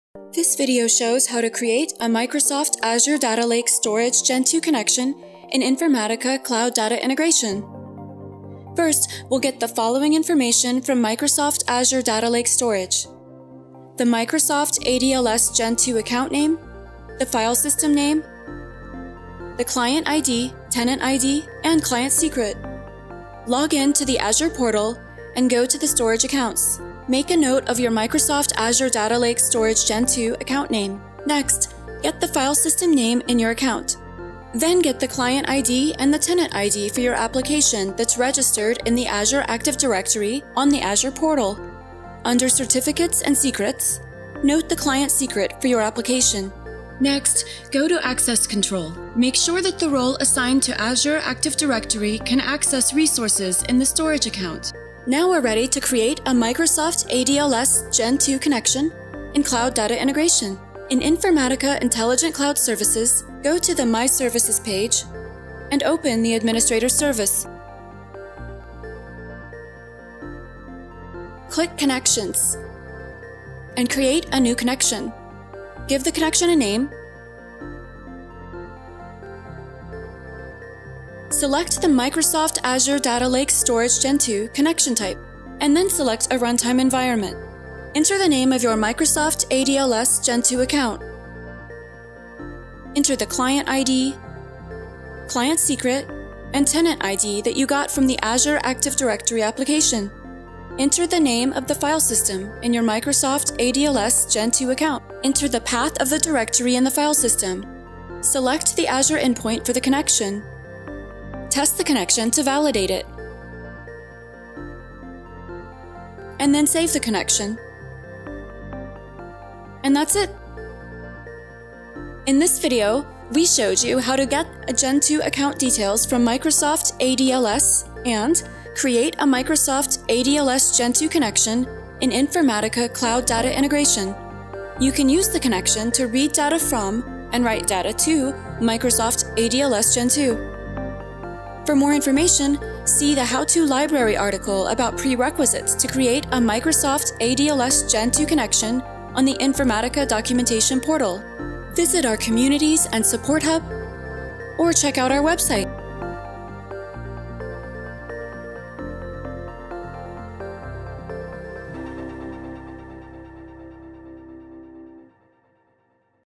Creating a Microsoft Azure Data Lake Storage (ADLS) Gen2 Connection in Data Integration
Learn how to configure a Microsoft ADLS Gen2 connection in Data Integration. This video is Part 1 of 3. Check out the other videos in this series: